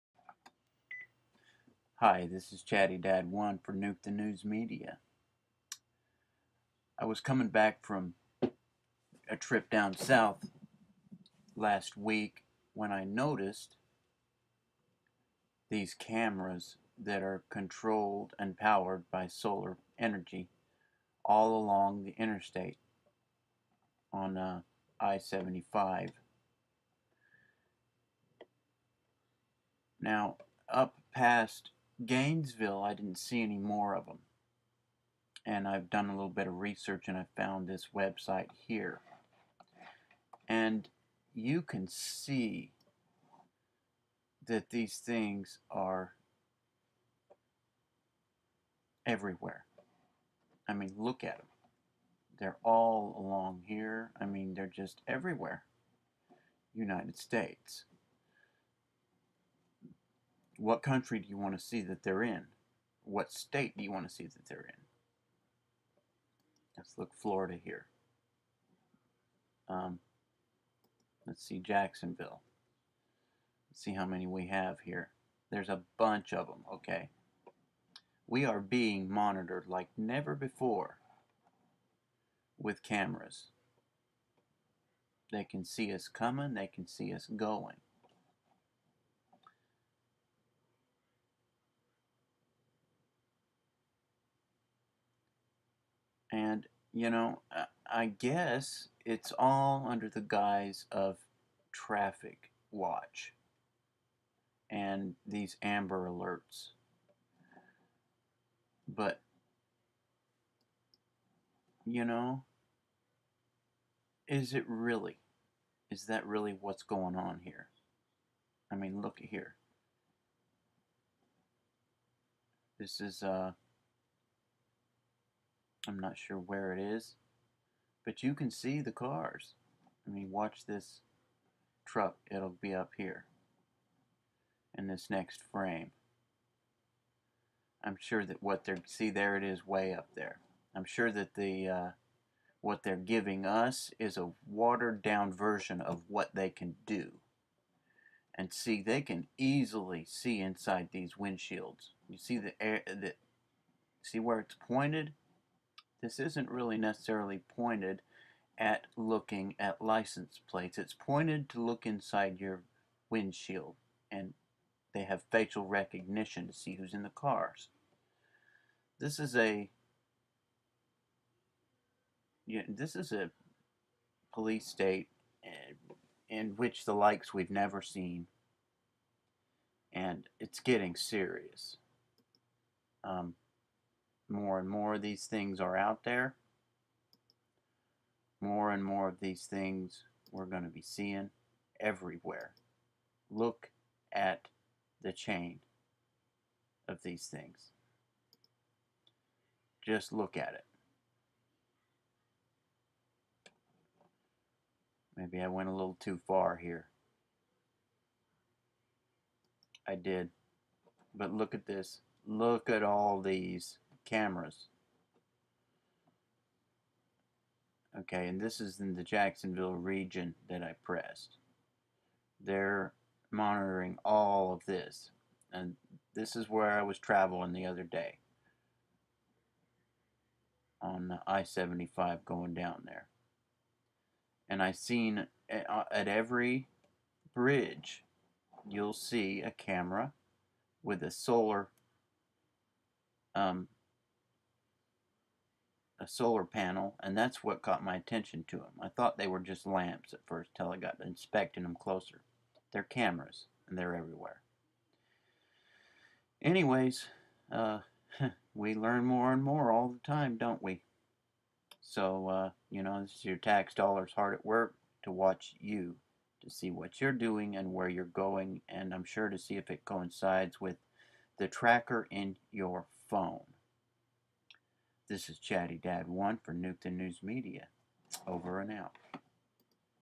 PRIVACY ALMOST GONE!! US INTERSTATE CAM SYSTEMS.WHY/WHO/WHEN?TRAFFICLAND.
from nukethenewsmeedia channel If you still think you have privacy,think again.You would have to go off the grid and underground to have complete privacy.Privacy is becoming a luxury for the powers that be,and even they cannot escape the watching eyes. Welcome to the NWO!!